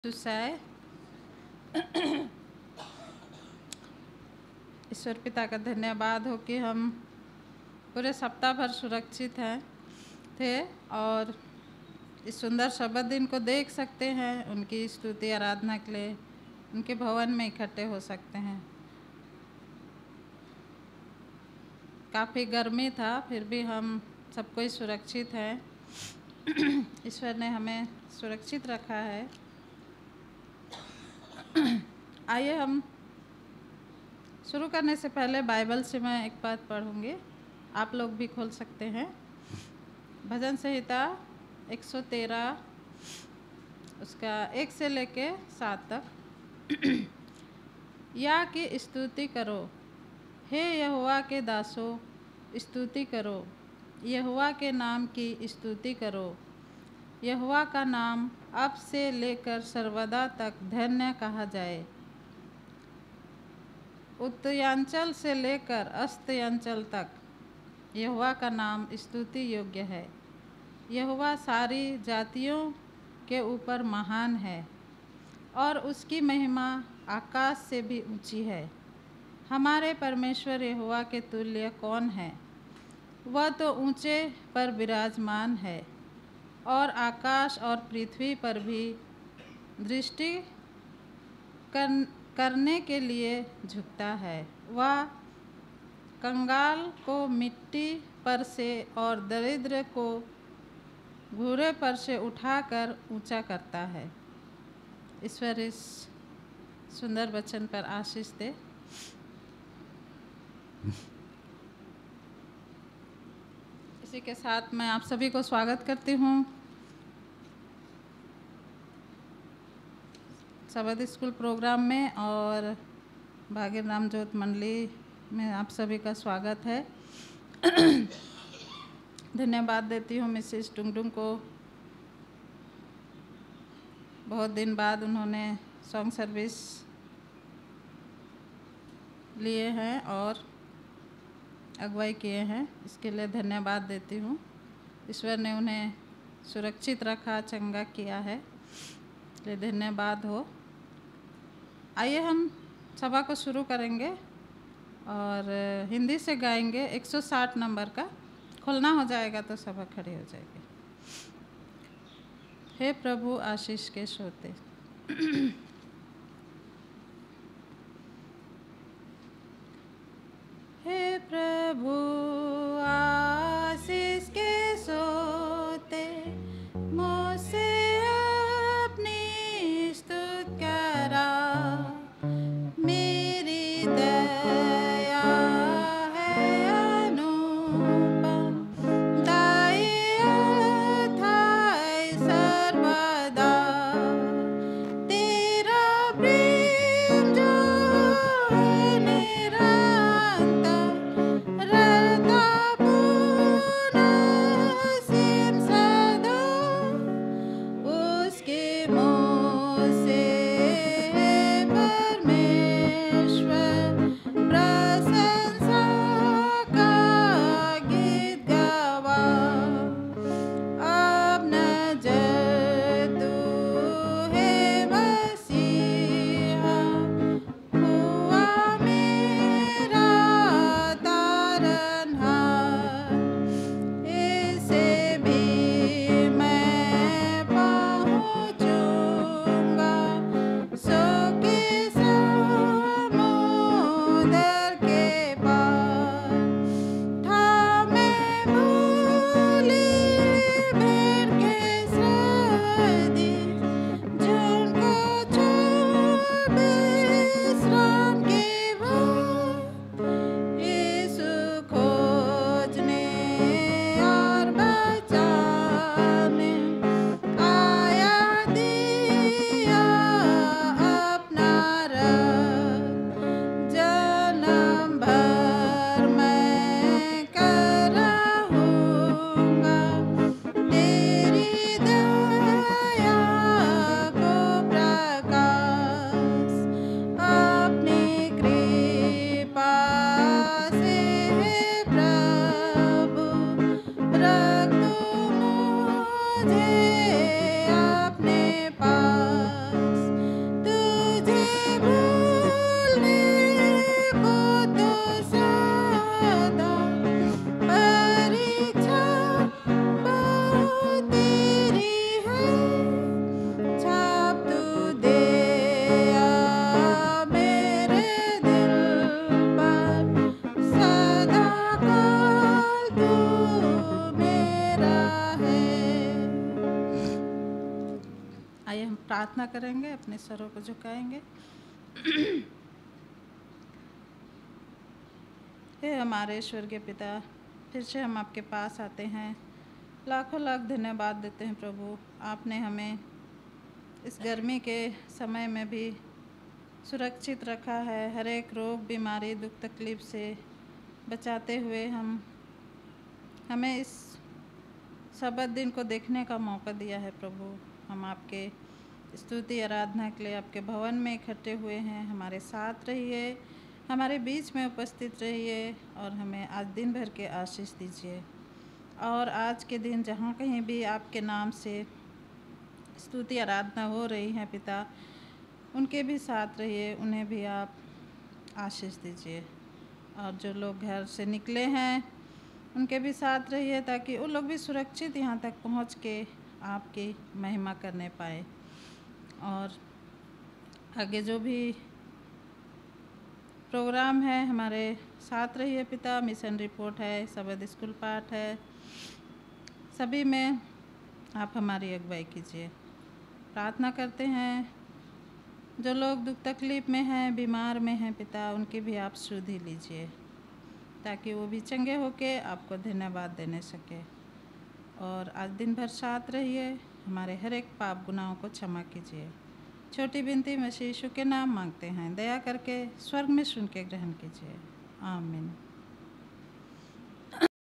"इंजील, स्तुति और प्रार्थना" - Mrs. Alista Salt || Sabbath: 10 June 2023
"सच्चे मनुष्य पर बहुत आशीर्वाद होते रहते हैं, परन्तु जो धनी होने में उतावली करता है, वह निर्दोष नहीं ठहरता।" - नीतिवचन / Proverbs 28:20
- It brings blessing to read the Scriptures
- Spend time in doing so.
- Be blessed as you watch the video and do share with others.